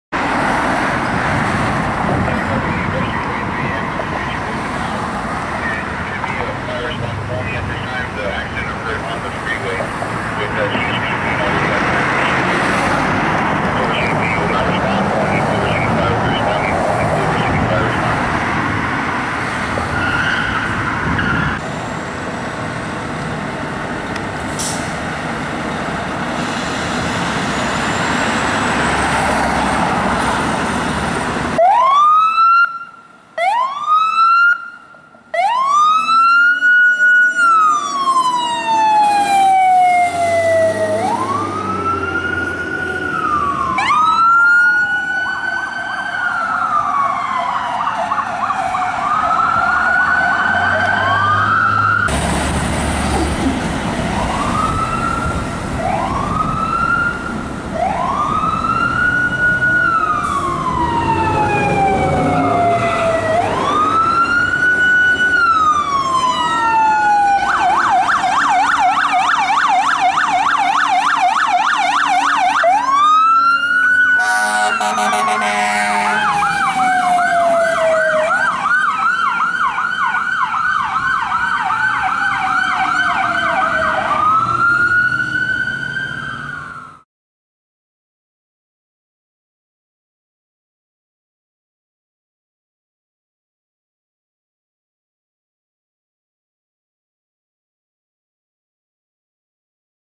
Culver City Fire Dept. Engine 42 & Ambulance 42
Culver City Fire Department:
Engine 42 and Ambulance 42 responding to a traffic collision on Sawtelle Blvd. and Culver Blvd.

Units due: Engine & Ambulance 42, Rescue 43 (canceled), CHP.